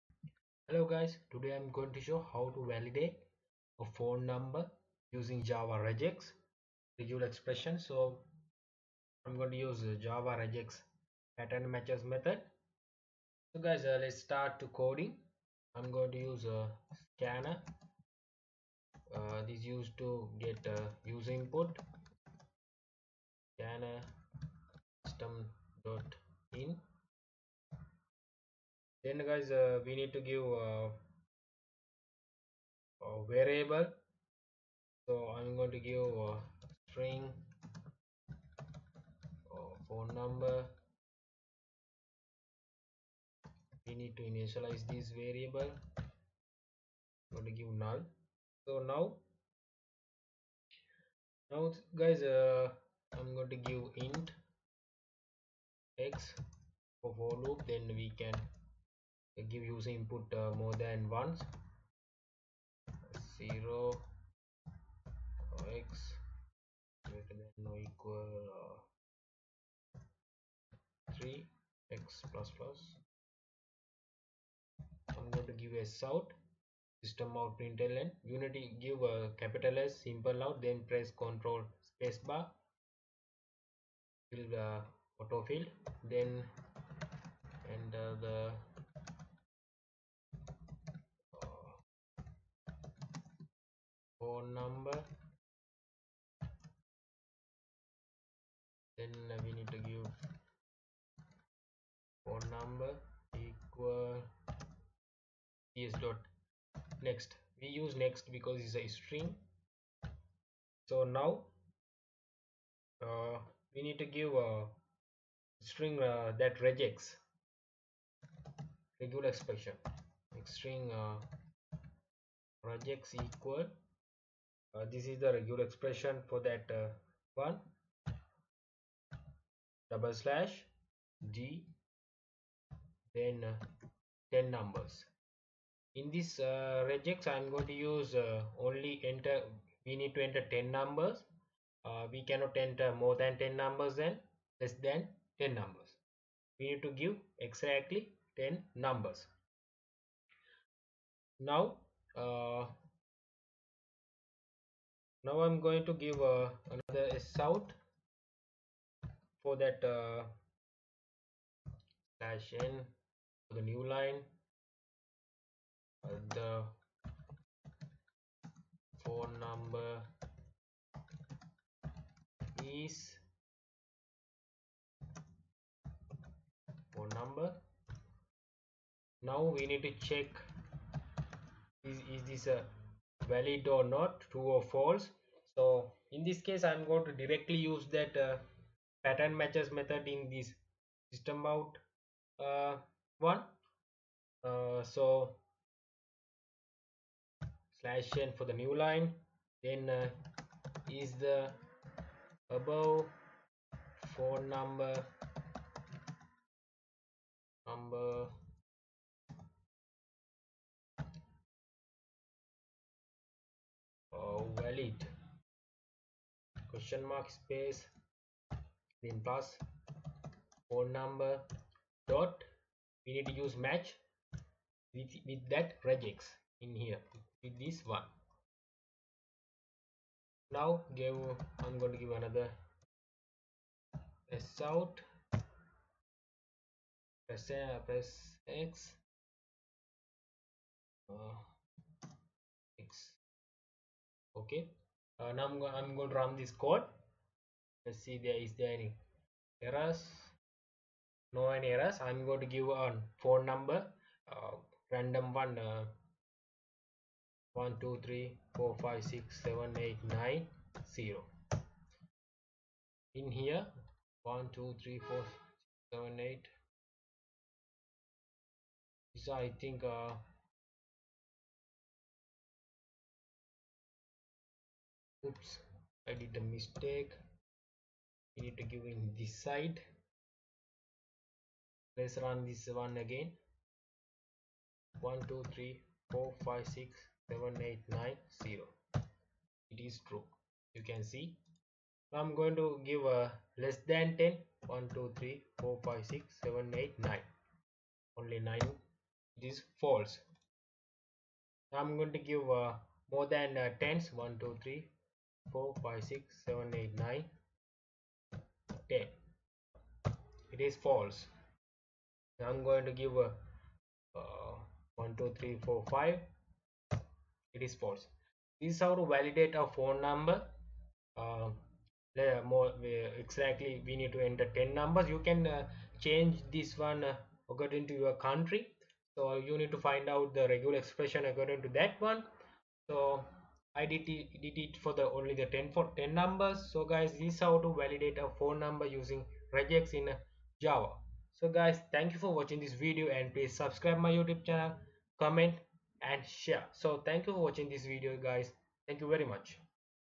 How to Validate Phone Number Using Java language | Validate Phone Number in Java
In this video I am going to show, how to Validate Phone Number Using Java language | Validate Phone Number in Java.